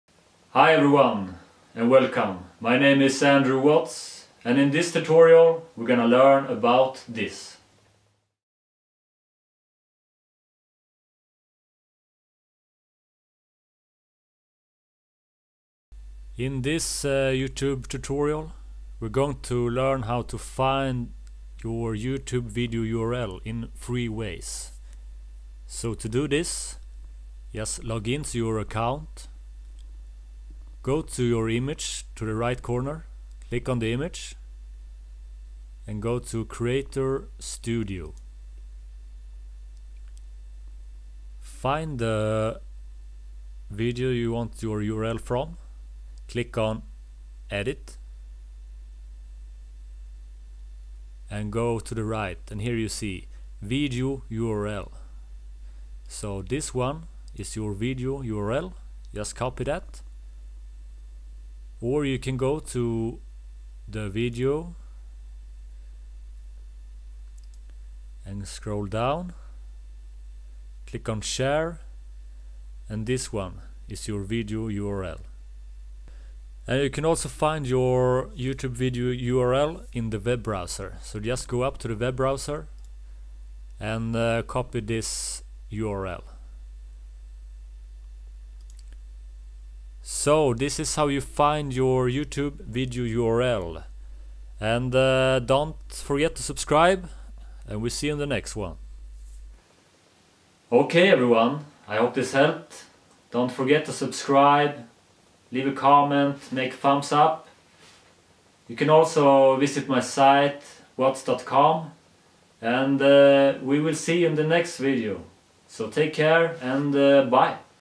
How to find YouTube video url - 2016 Tutorial
How to find YouYube video url fast and easy. This is a great way to share your videos and promote to get more views.